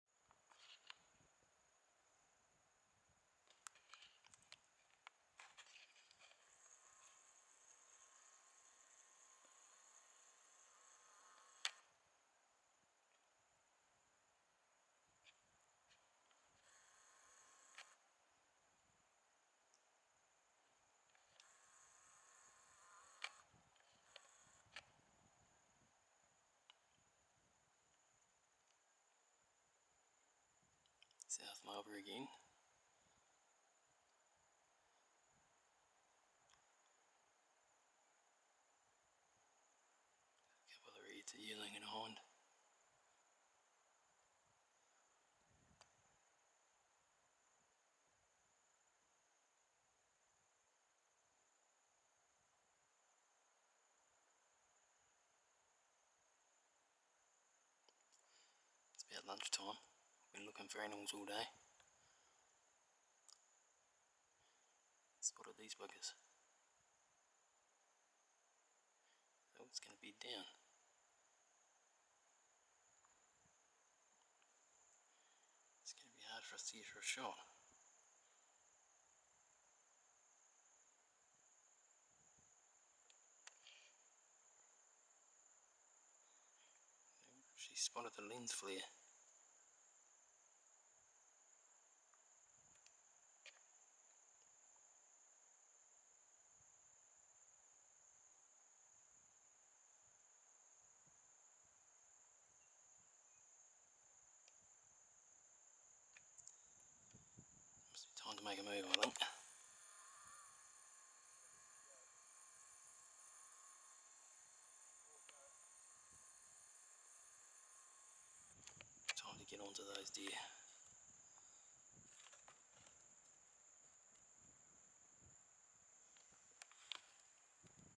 Red Deer South Marlborough Winter.
Spotted these deer up high on the colder side of the valley after glassing for hours on the warmer sunny faces. Plan was to take the yearling but also spotted a stag with an ugly 6 point head who after going undetected for half the stalk had us pinned down with his 1000 yard stare for 15mins after my mate spotted him. But he soon lost interest and we got into position half an hour later and myself and my mate took both animals. Left the hind the have her unborn fawn and hopefully a stag with trophy potential will move into the area and breed. We covered 18km that day and got back to the hut at 9:00pm that night.